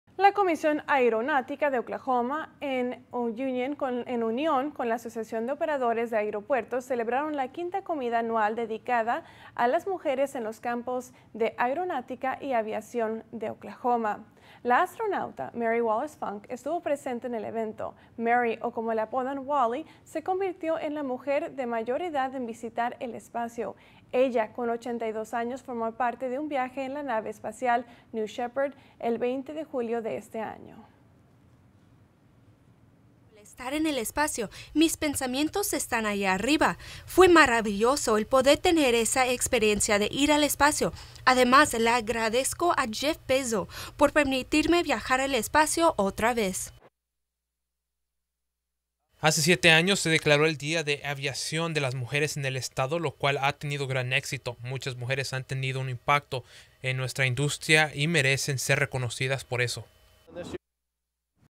Mujeres en los campos de aeronáutica y aviación
La semana pasada la comisión aeronáutica de Oklahoma en unión con la asociación de operadores de aeropuertos celebraron el día de las mujeres en los campos de aeronáutica y aviación.